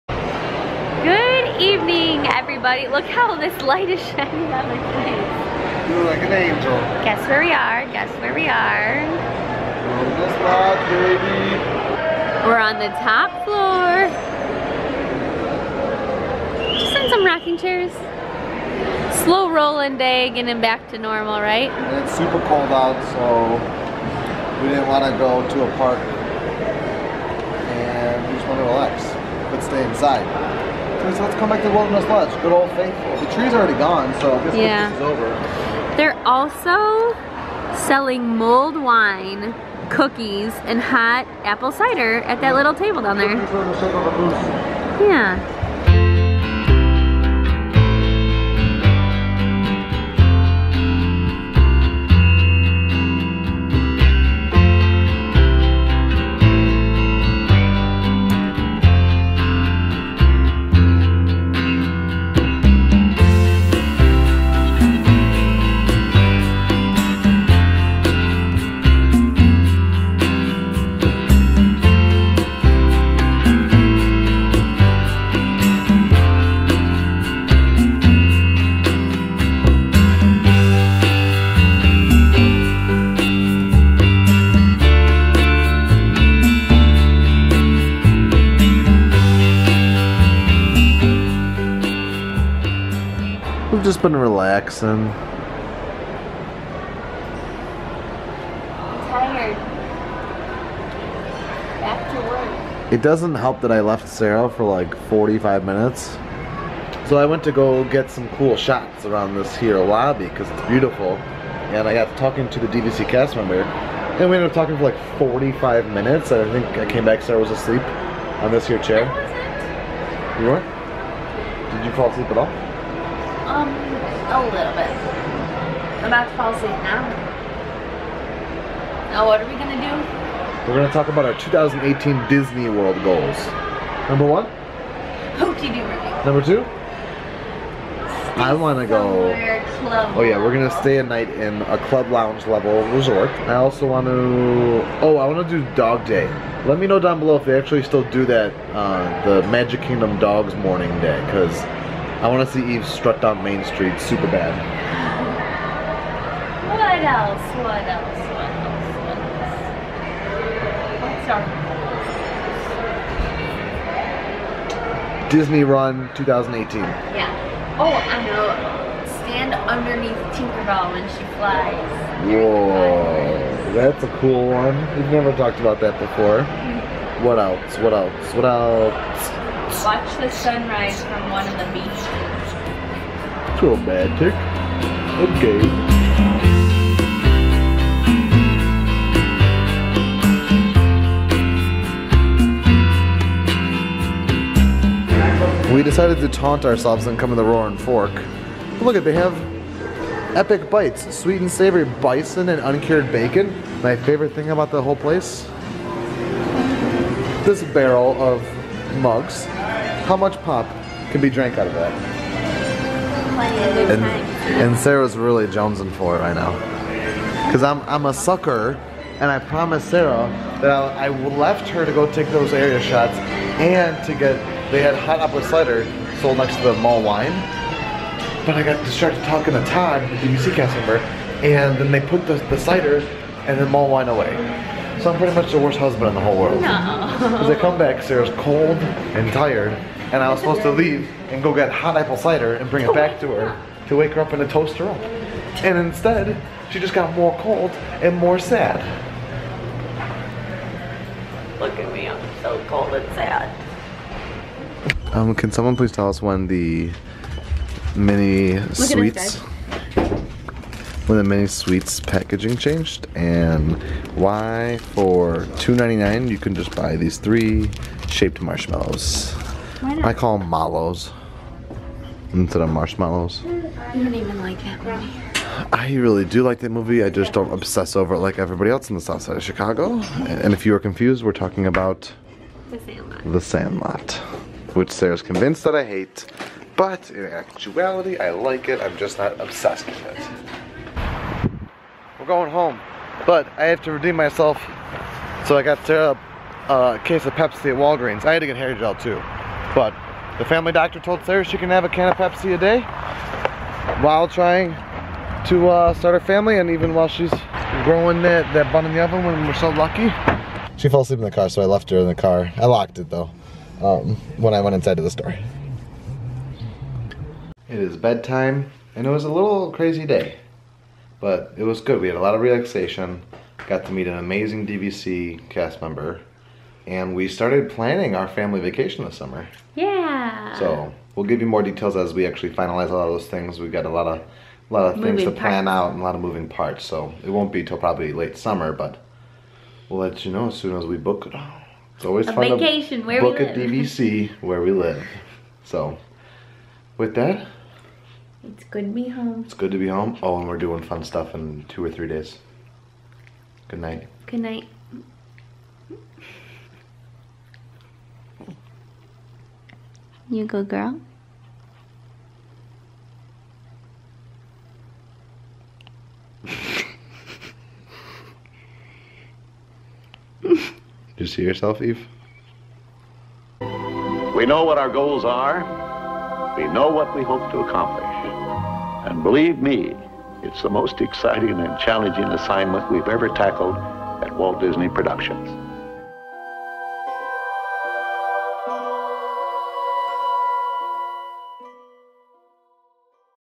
An Ode to the Wilderness Lodge Lobby
We went back to work today... and are pretty worn out from it! We take to the Wildness Lodge for a relaxing evening, appreciating its glorious lobby. We talk about our 2018 goals and even our family vacation to take place this year! Eeeeek!